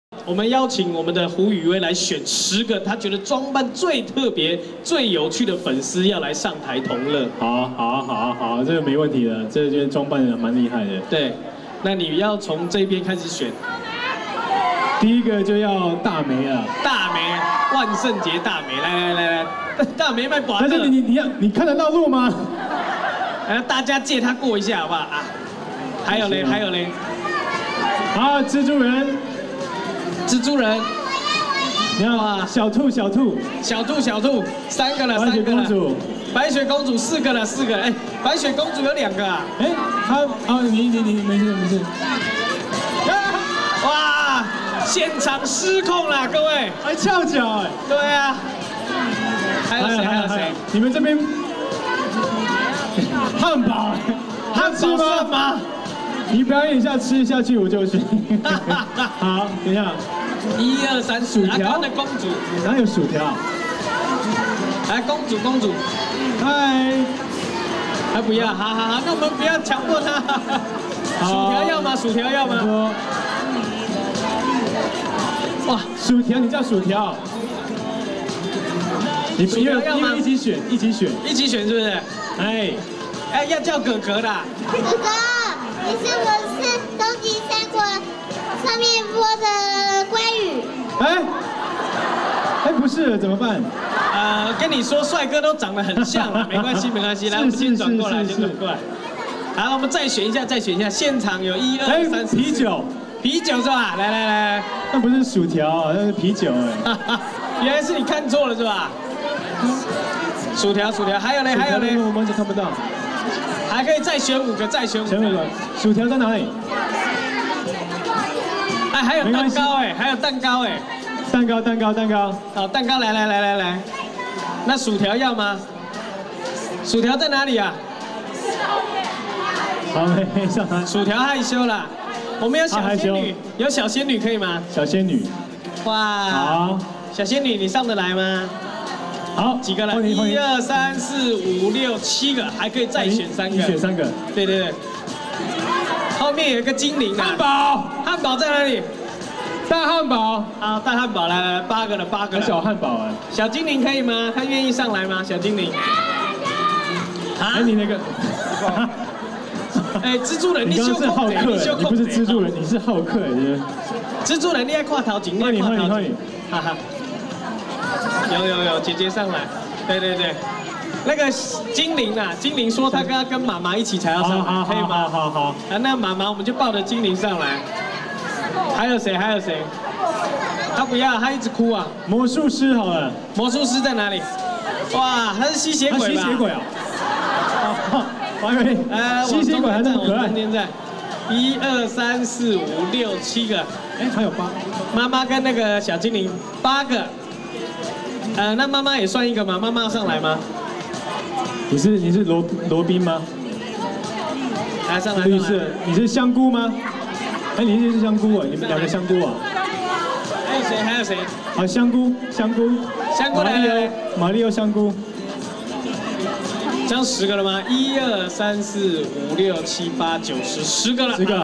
胡宇威 挑選裝扮最特別的粉絲
食破天驚2 胡宇威配音記者會，胡宇威 挑選裝扮最特別的粉絲。